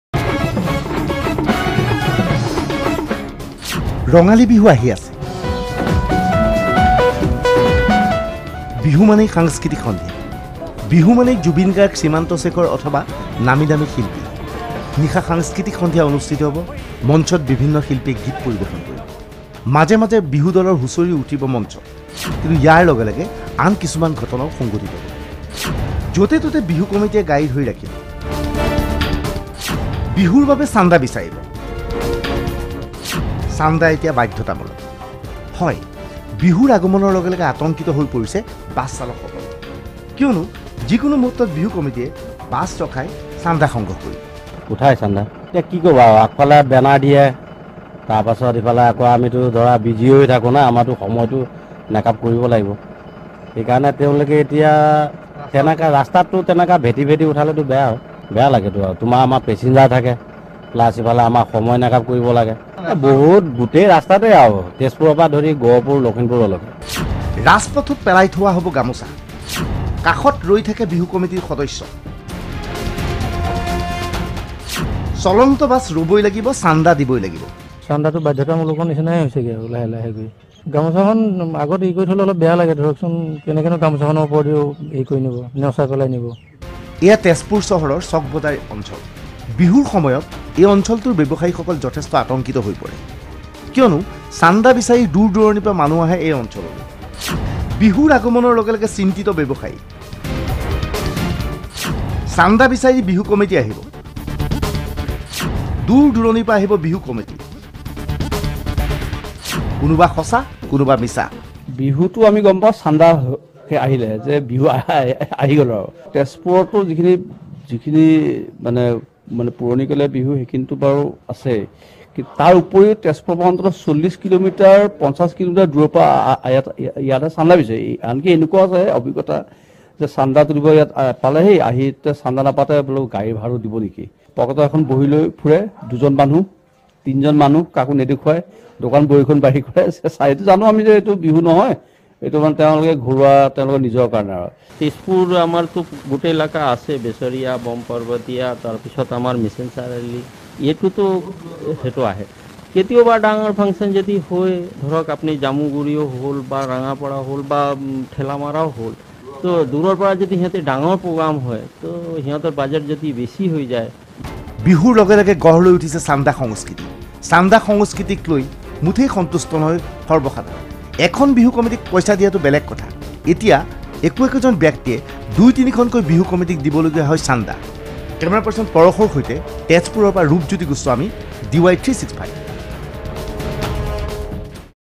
বিহু সংস্কৃতিৰ লগে লগে চান্দা সংস্কৃতি || Bohag Bihu committee & Bihu Chanda/Donation
ৰাজ্যখনত বিহু সংস্কৃতিৰ লগে লগে গঢ় লৈ উঠিছে চান্দা সংস্কৃতি। বিহুৰ আগমনত অসমবাসী উত্সাহিত যদিও চান্দা সংস্কৃতিক লৈ চিন্তিত একাংশ। বিশেষকৈ ব্যৱসায়ী মহল আৰু বাছচালকসকল বিহু আগমনৰ লগে লগে চিন্তিত হৈ পৰিছে।